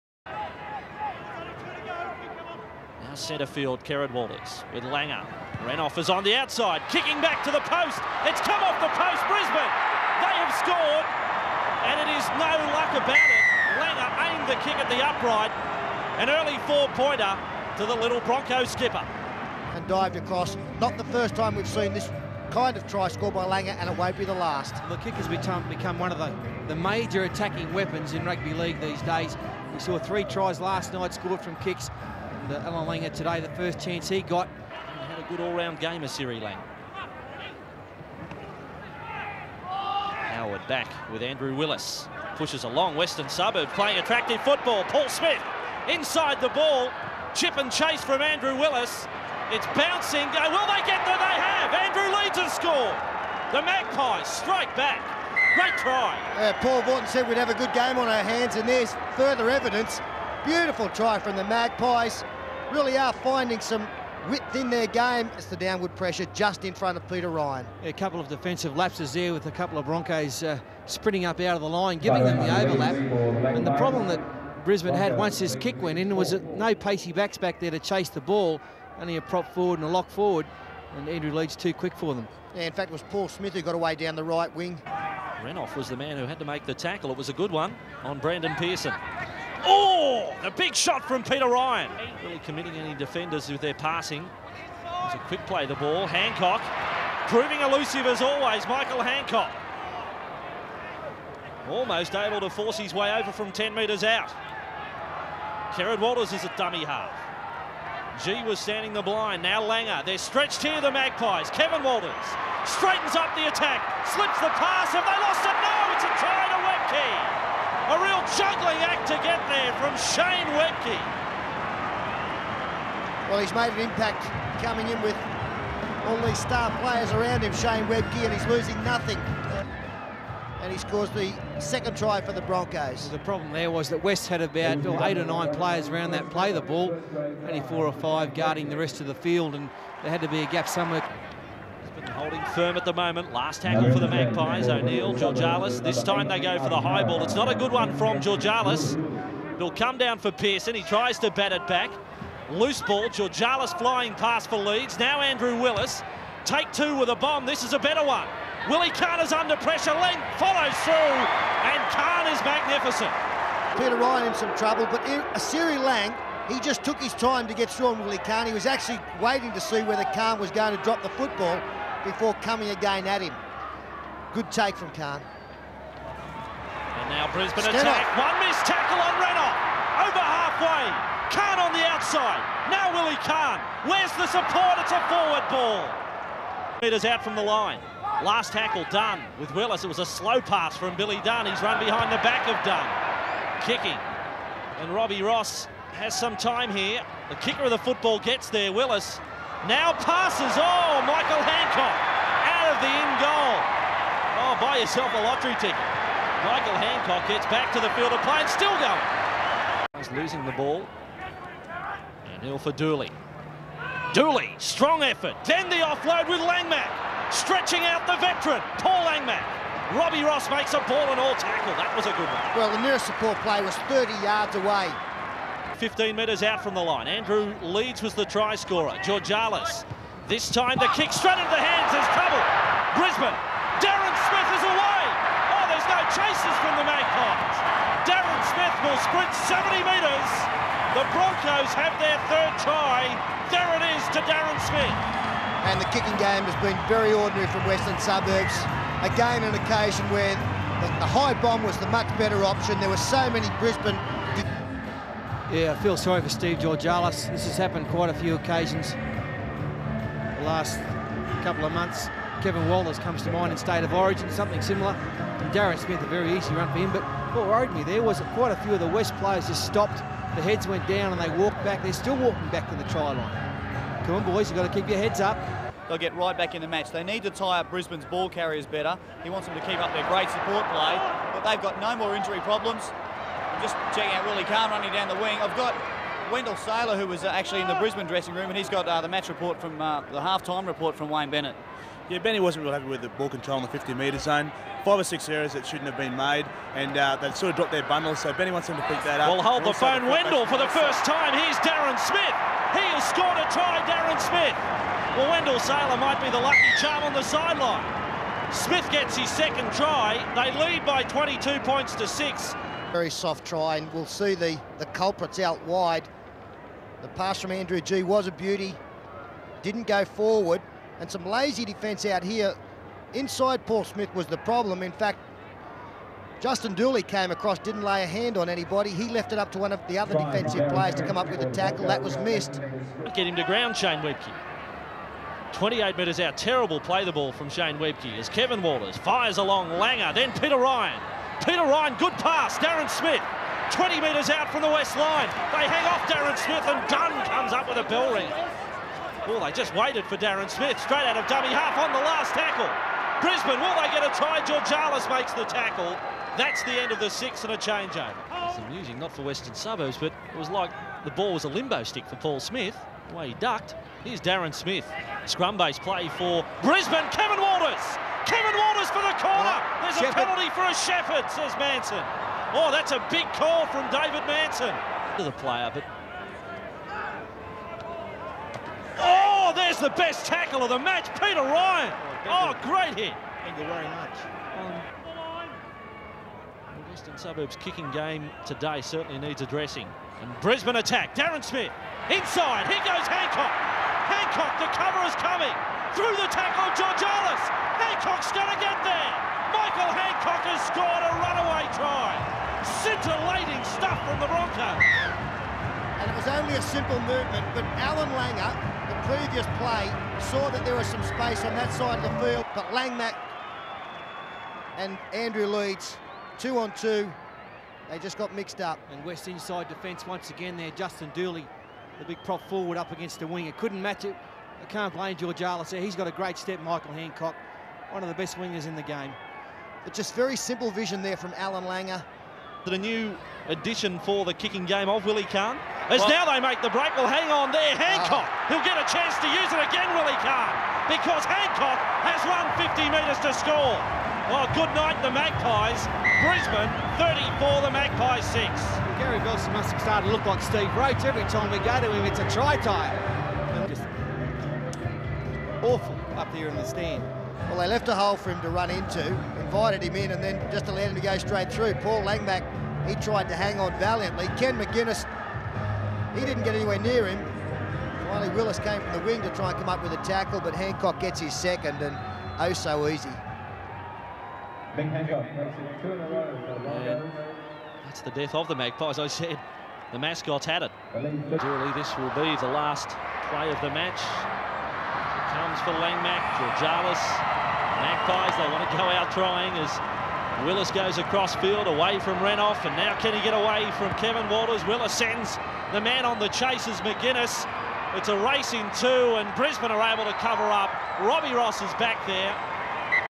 Brisbane Broncos v Western Suburbs Magpies | Round 19, 1996 | Condensed Match | NRL
The Broncos host the Magpies during the 1996 season.